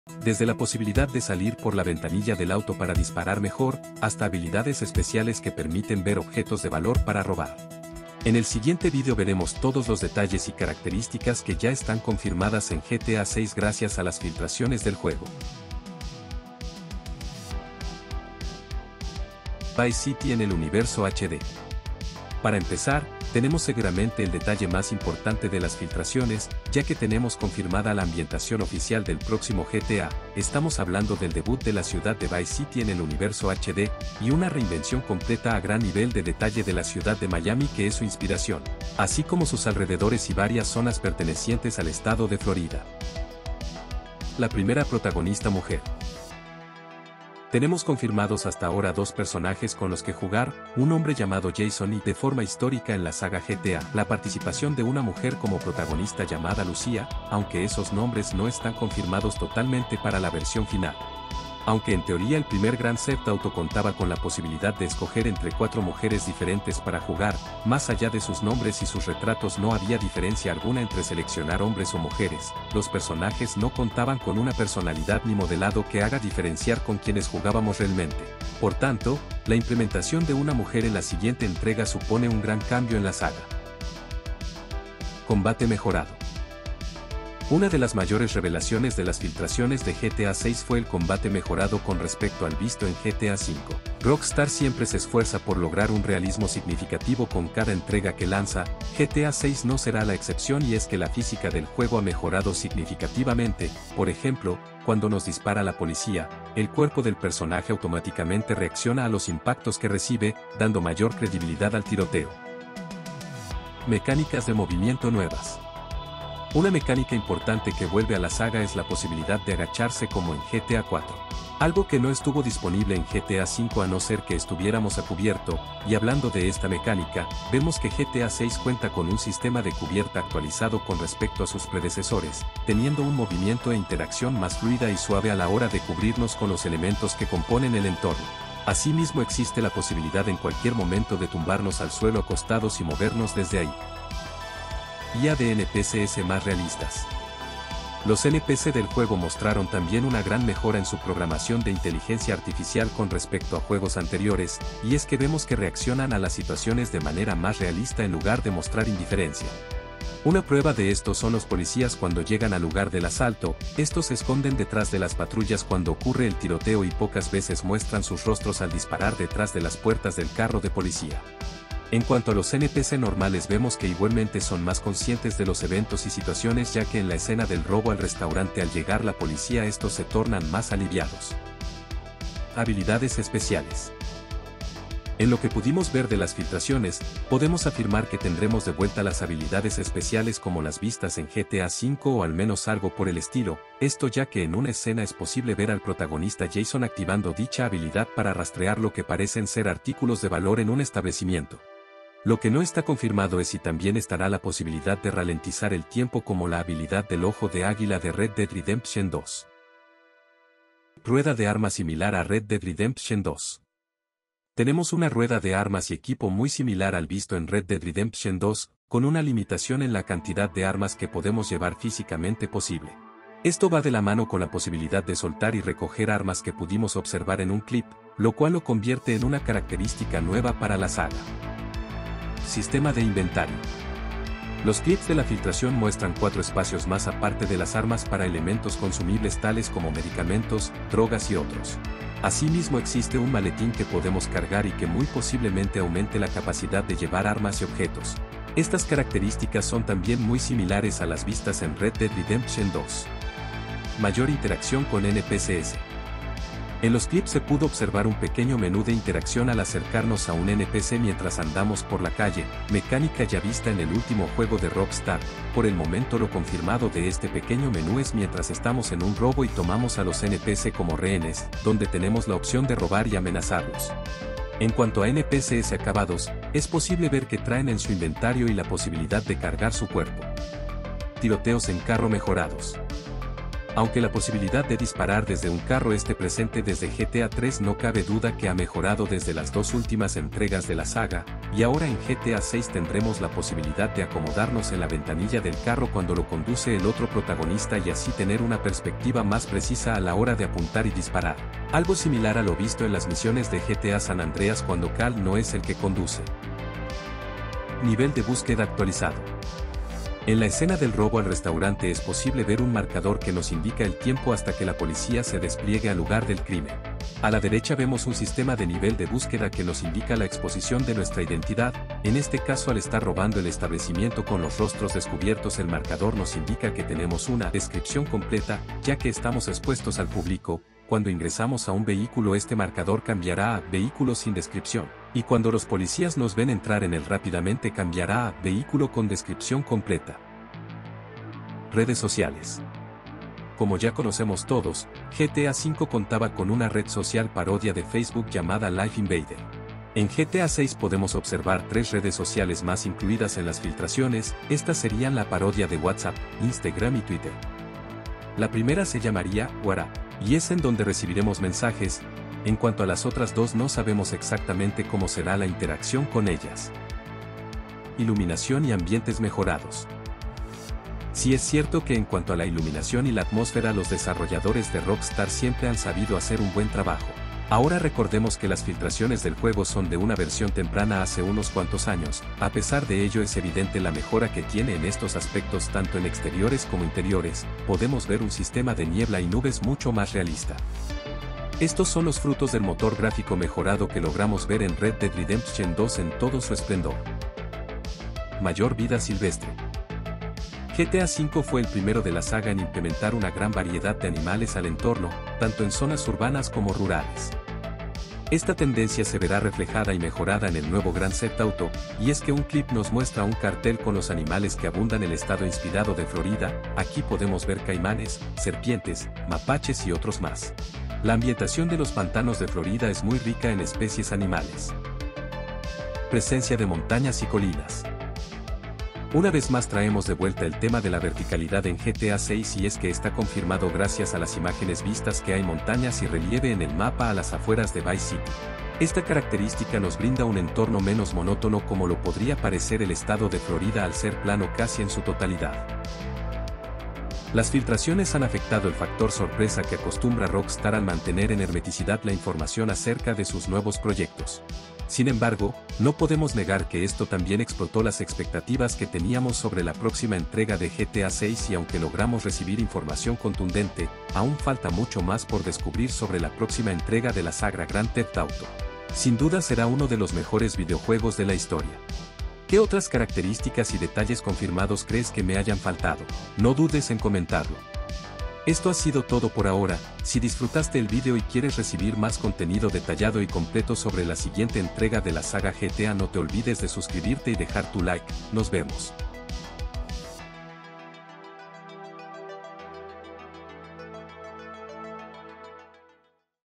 Todos los Detalles y Características Confirmados en GTA 6
Hola qué más amigos, sin duda alguna todos estamos emocionados por la próxima entrega de la saga Grand Theft auto, y es que las filtraciones solo lograron que nuestras expectativas aumentaran mas por todos los detalles y características que pudimos observar y analizar. En este video me voy a ceñir a todo lo que está realmente confirmado gracias a las filtraciones, obviando especulaciones y teorías surgidas aún no resueltas.

Es posible que algunas características de la fase temprana del juego sufran cambios en la versión final, sin embargo la base de esos detalles está fuertemente cimentada en lo que mencionamos en el video.

Momentos del video:
0:00 Inicio
0:19 Vice City en el Universo HD
0:44 La primera protagonista Mujer
1:25 Combate mejorado
1:53 Mecánicas de movimiento nuevas
2:25 Inteligencia Artificial de NPC's más realista
3:02 Habilidades especiales
3:31 Rueda de armas como RDR 2
3:52 Sistema de inventario
4:13 Mayor interacción con NPC's
4:41 Tiroteos desde el carro mejorados
5:07 Nivel de búsqueda actualizado
5:41 Redes sociales
6:07 Iluminación y ambientes mejorados
6:37 Mayor vida silvestre
7:06 Presencia de Montañas y colinas
7:27 Conclusión y despedida

Saludos!